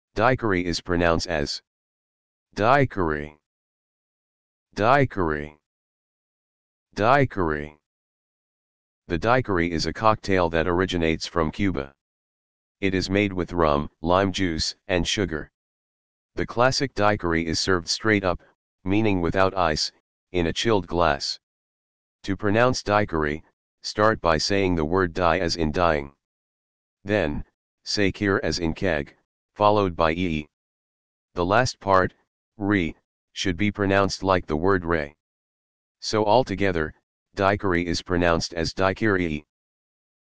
How To Pronounce Daiquiri
The daiquiri is a cocktail that originates from Cuba. It is made with rum, lime juice, and sugar. The classic daiquiri is served straight up, meaning without ice, in a chilled glass.

To pronounce daiquiri, start by saying the word "die" as in "dying." Then, say "keer" as in "keg," followed by "ee." The last part, "ree," should be pronounced like the word "ray." So altogether, daiquiri is pronounced as "dye-keer-ee."